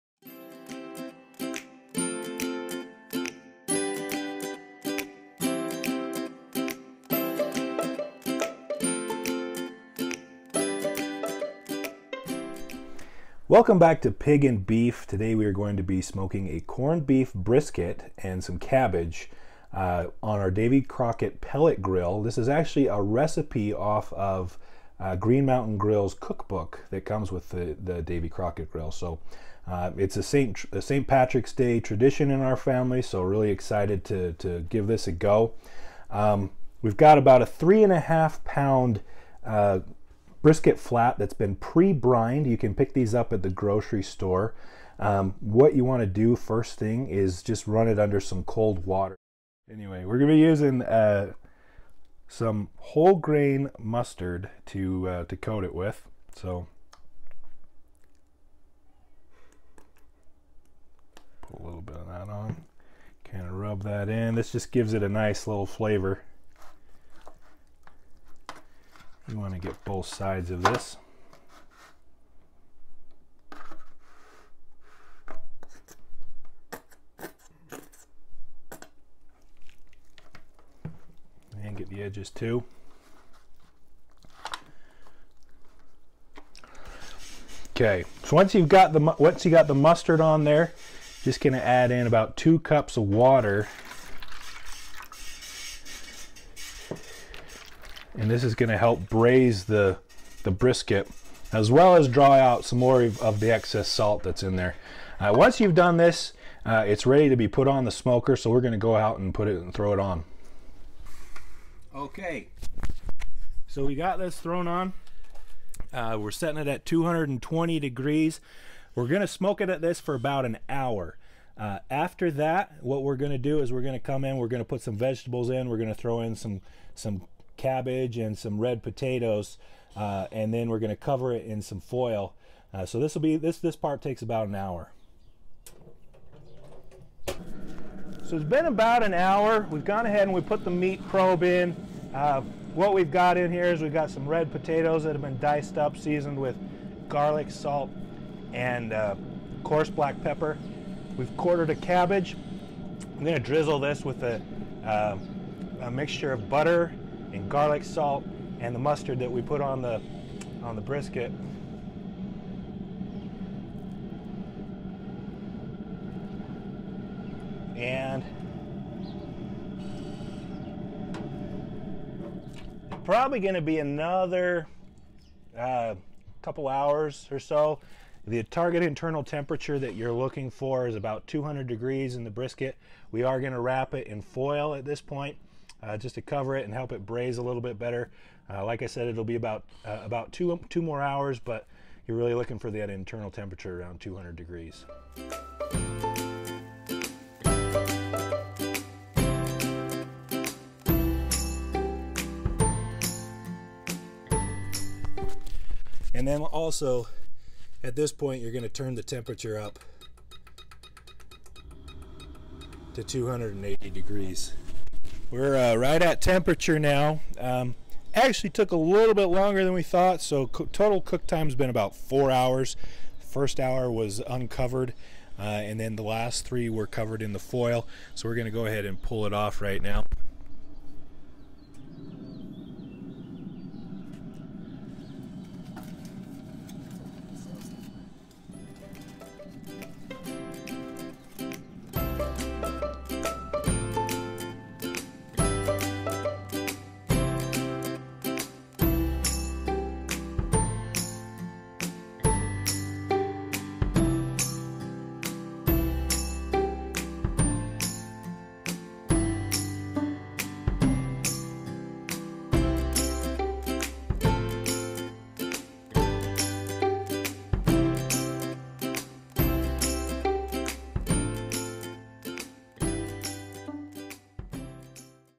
Corned Beef & Cabbage - Smoked & Braised on a Wood Pellet Grill
Learn how to make smoke and braise corned beef and cabbage on a wood pellet grill!  This is a delicious twist on corned beef and adds a hint of smokey flavor.  This recipe is courtesy of Green Mountain Grills.

Prep Time: 5-10 minutes
Cook Time: Approx. 3-4 hours
Grill: Green Mountain Grills Davy Crockett Grill
Ingredients: Corned beef, 2 cups water, 6 red potatoes, 1 head cabbage, 3 tsp. garlic salt, 1 tsp. ground black pepper, 3-4 tsp. whole grain mustard, 3 tbsp. melted butter

Step 1: Rinse the corned beef under cold running water to remove excess salt.  Coat both sides with 2-3 tbsps. of the mustard.

Step 2: Add the corned beef and water to an aluminum pan and smoke at 220, uncovered, for one hour.

Step 3: Quarter the cabbage, removing the stem and core.  Melt the butter and stir in 1 tbsp. mustard and 1 tsp. garlic salt.  Place the cabbage quarters in the aluminum pan in each corner, side up to act like bowls.  Drizzle the cabbage with the melted butter mixture.

Step 4: Cut the potatoes in half and season with 2 tsps. of the garlic salt and 1/2 tsp. of the pepper.  Place along the edges of the aluminum pan between the cabbage quarters.

Step 5: Cover tightly with aluminum foil.  Turn the grill up to 280 and cook about two to three more hours until the internal meat temperature reaches 200-205 and the vegetables are tender.  Remove the foil and cook 14 more minutes.